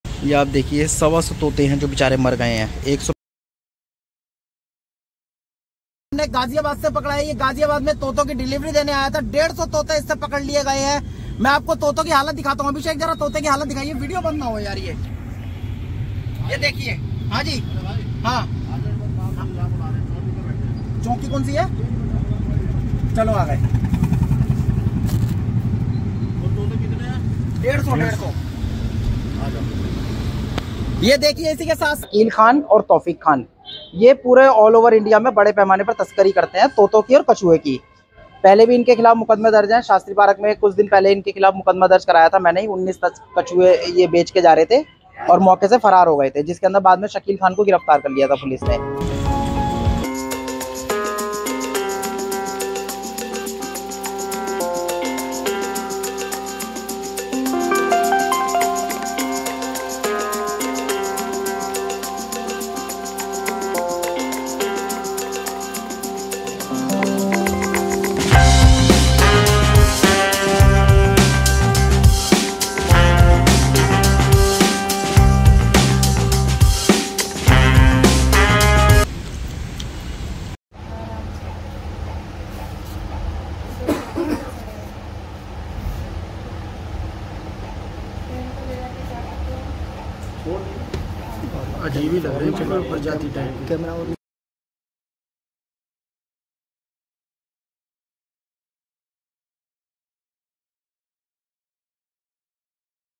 पक्षियों को मुक्त कराया| parrots rescued from ghaziabad
पुलिस पर हमला | PFA ने जान पर खेलकर पक्षियों को आजाद कराया